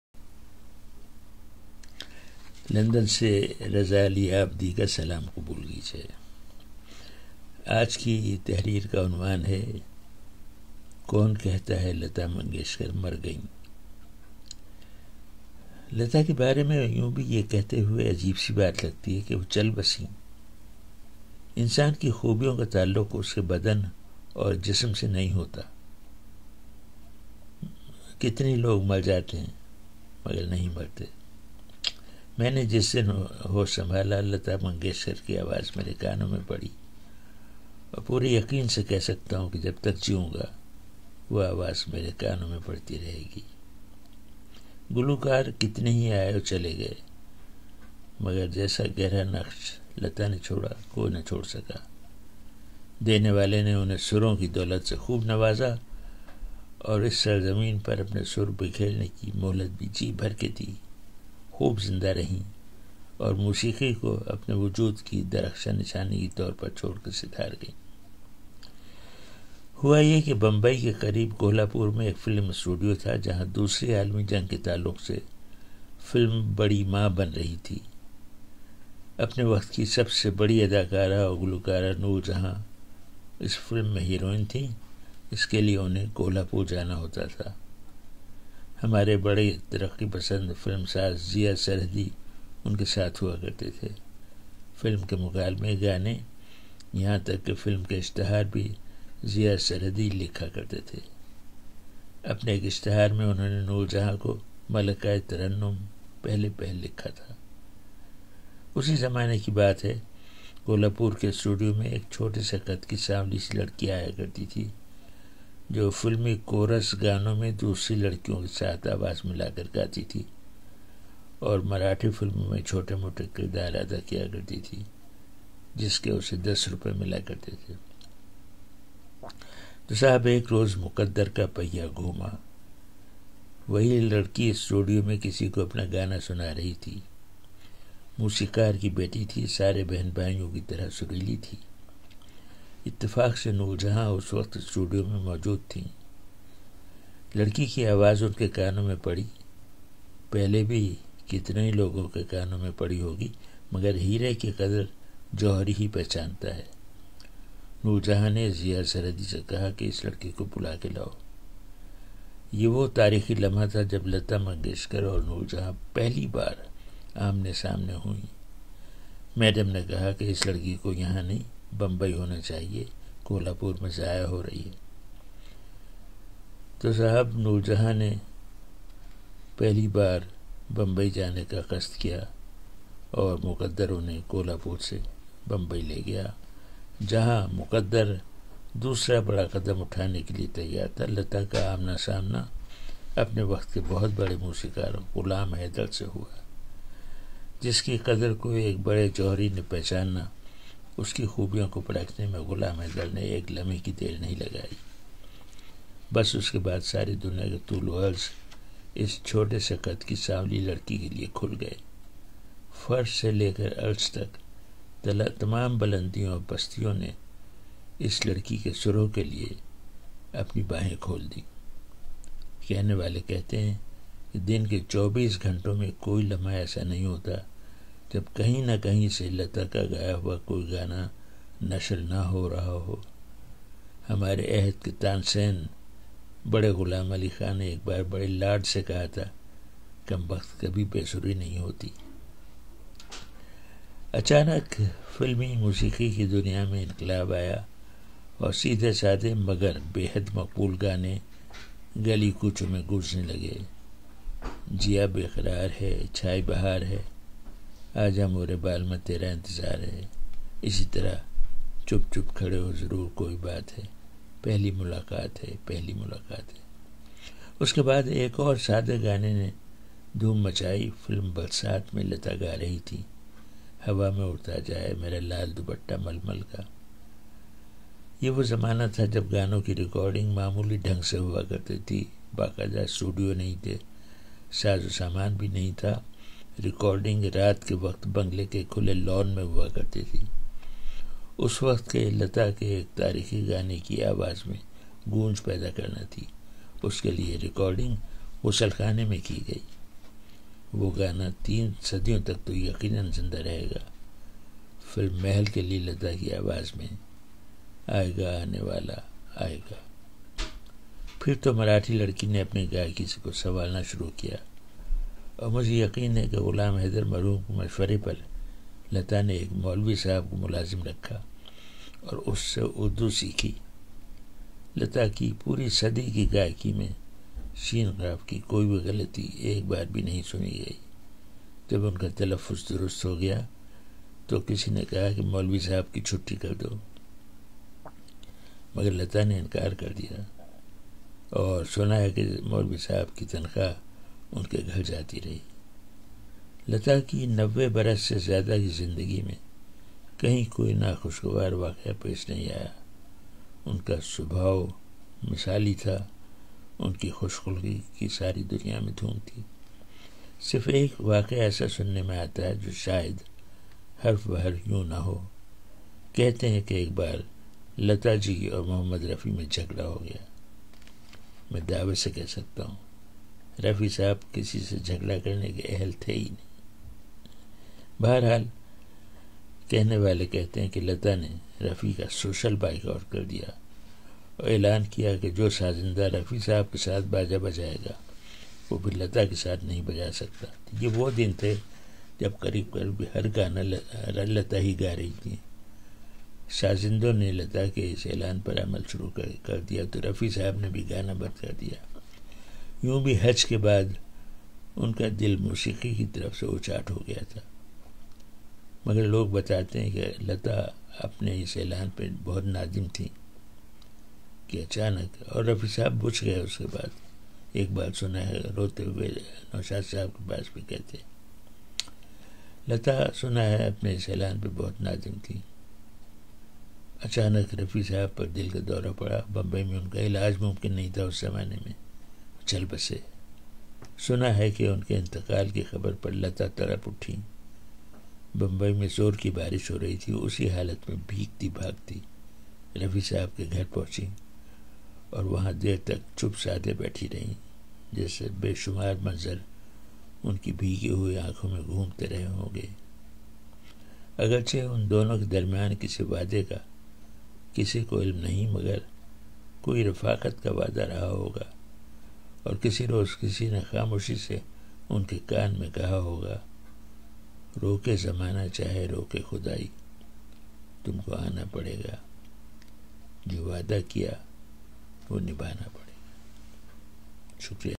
कौन कहता लता मंगेशकर मर गईکون کہتا ہے لتا منگیشکر مر گئیں
लंदन से पत्रकार रजा अली आबिदी अपने ताजा लेख mey लिखते हैं,कौन कहता है लता मंगेशकर मर गईं